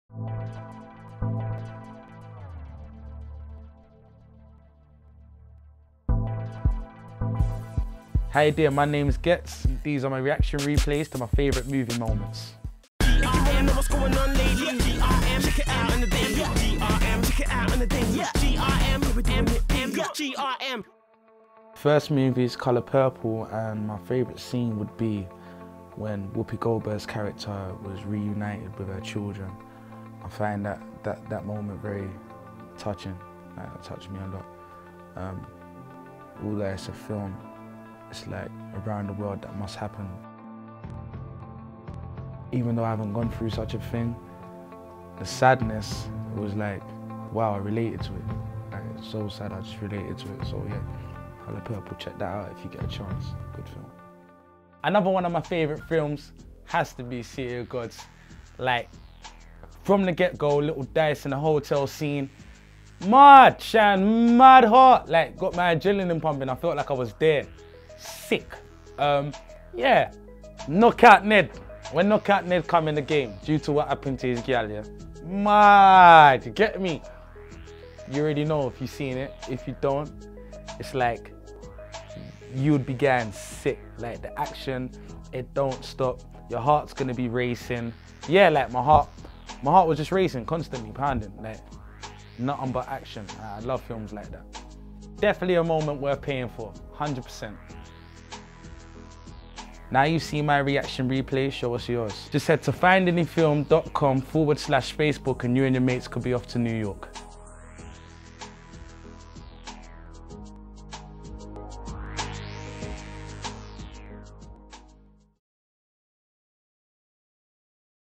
Ghetts ReactionReplay
Check out Ghetts' favourite movie moments for some inspiration! Do you remember your reaction to your favourite movie moment? Enter it into the Reaction Replay competition for your chance to win a trip to New York for you and three mates...